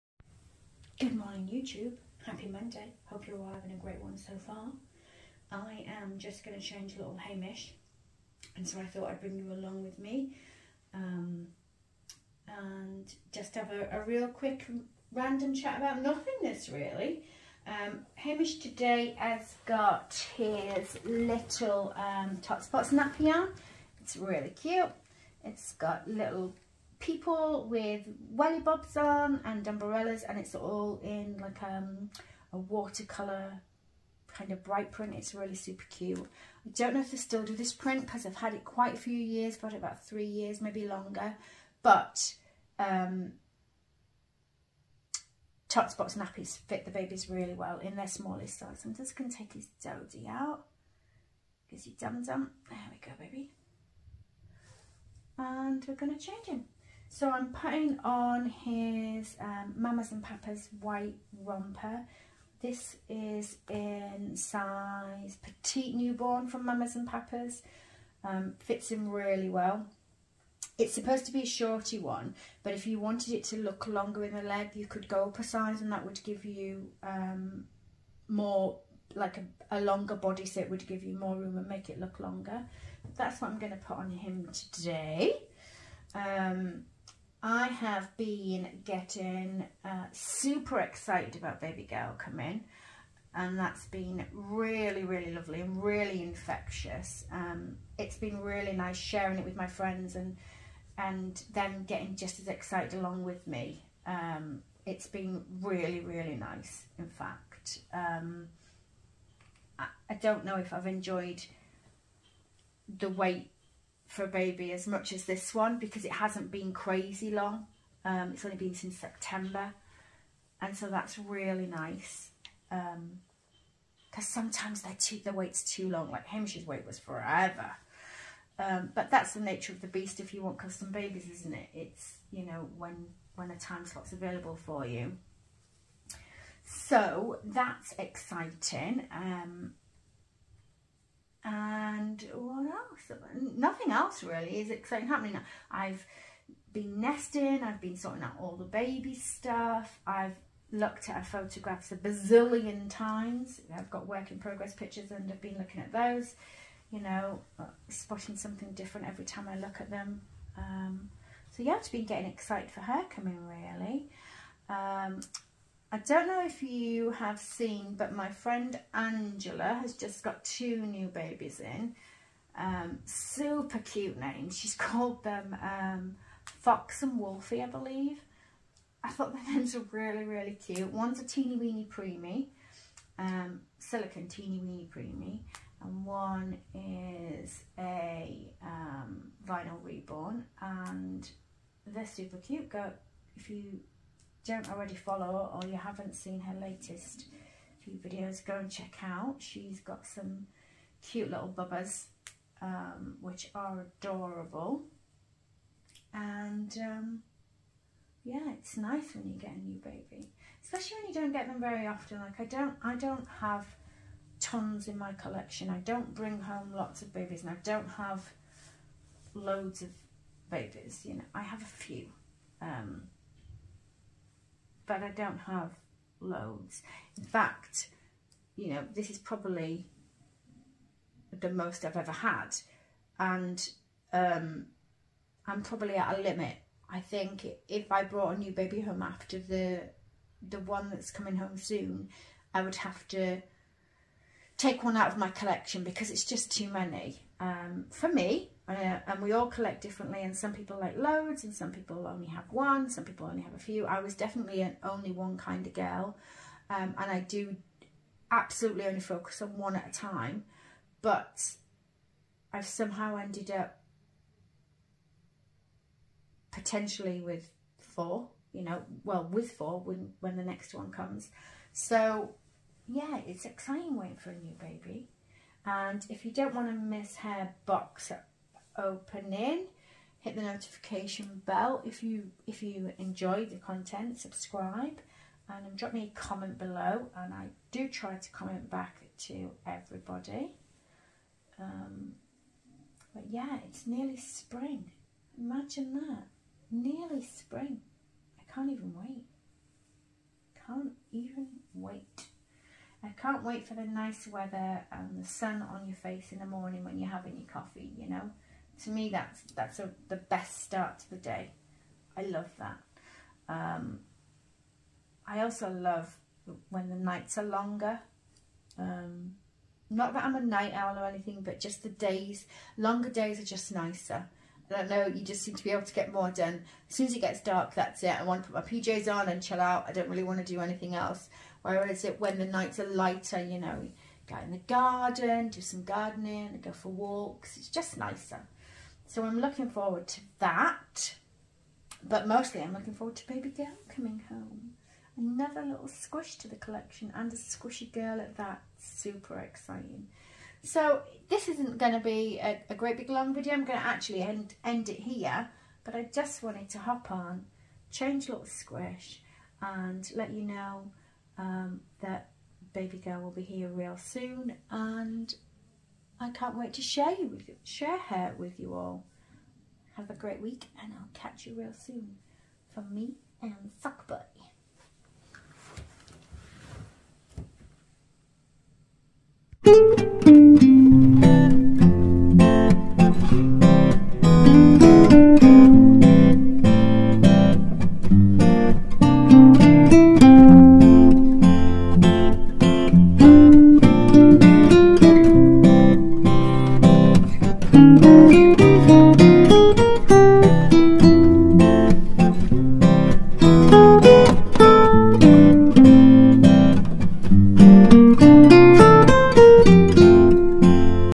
a little change.... Izzy Zhao silicone baby painted by Leticia Moreno😍
features Hamish full body silicone baby  to represent a newborn baby boy 'Bear' sculpted by Izzy Zhao @babynookartdolls painted by the wonderful Leticia Moreno @dreamsoftinyangels to see more of my babies, baby fashion and accessories please follow my Instagram ‎@MonAmi_Lei

As mentioned @Angelicdollsandbears  has some new little bubs   to check out 🤍